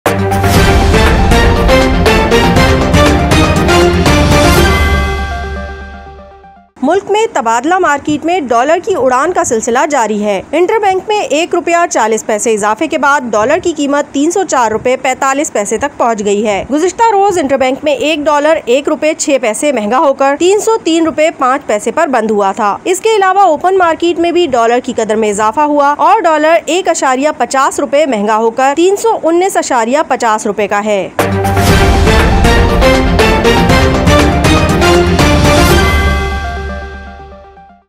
Depreciation of the rupee continued, dollar became 304 rupees 45 paise in interbank
Depreciation of the rupee continued, dollar became 304 rupees 45 paise in interbank | SuchExpressNewsOfficial